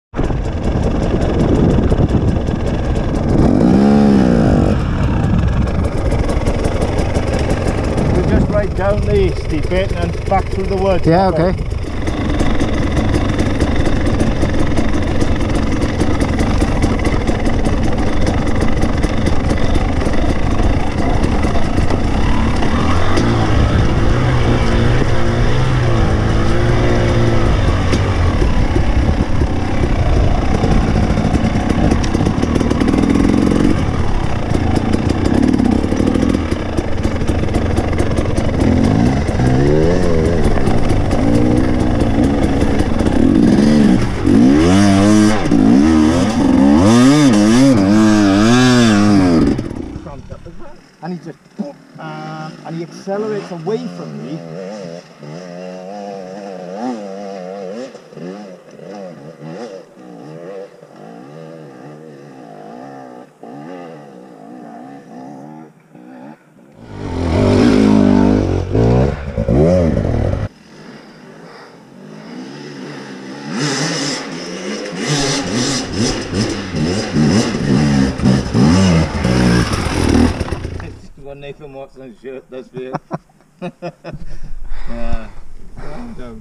2019 KTM 300 TPI and KTM 250 TPI's
Weekend ride in South Wales